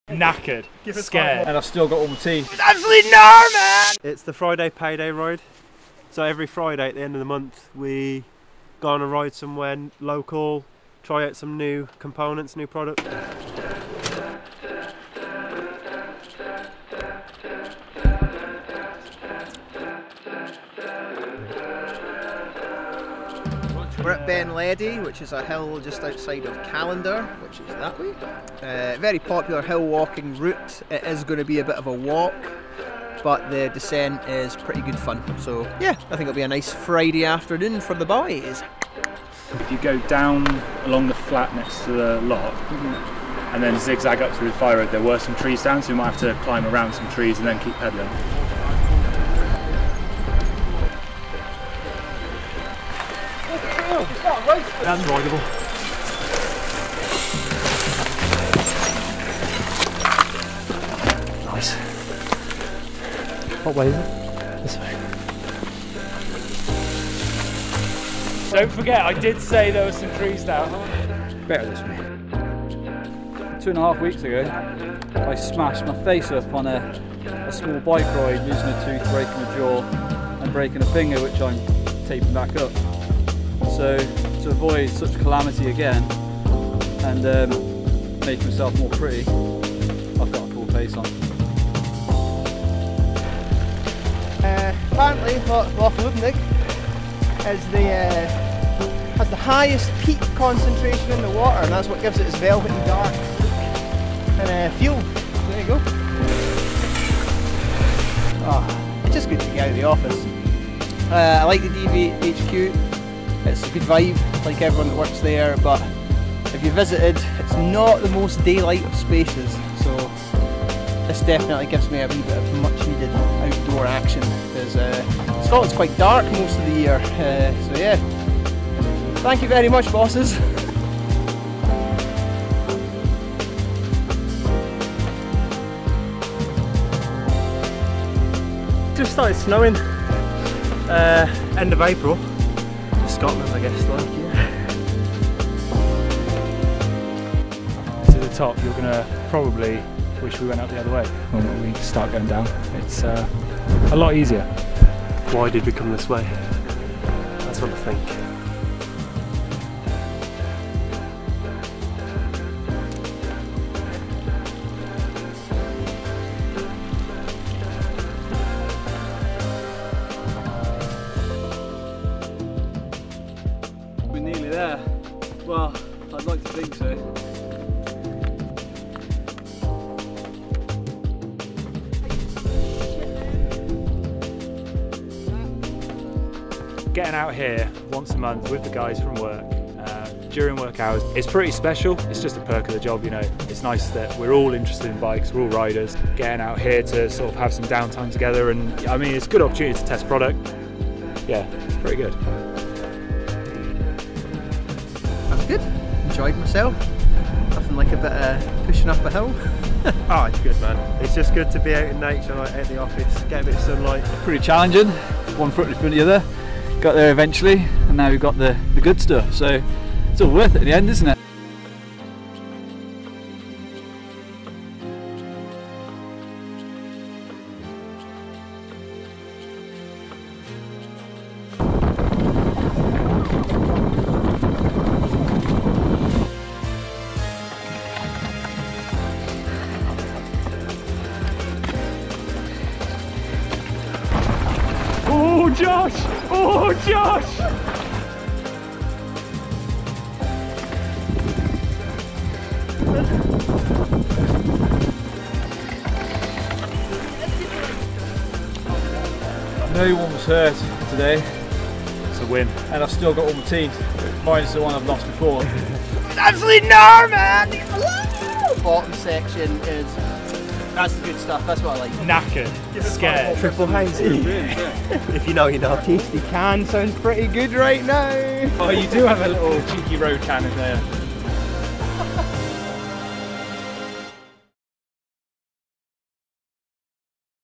Scottish Routes Episode 3: Ben Ledi | Deviate Cycles
Want to ride this trail too?

Join us for episode 3 of the Deviate Cycles Scottish Routes Series. The Deviate staff take us on a tour of one of their locals. Ben Ledi. With it's steep ascent, followed by a never ending descent full of fun, chunder, and flow. This is definitely a trail to consider if you haven't ridden it already!

Route Overview:
Average Duration: 1-2 Hours approx.

Distance: 12km (Route Dependant)

Elevation: 795M (Avg)

Technical Difficulty: Moderate

Required Fitness Level: Moderate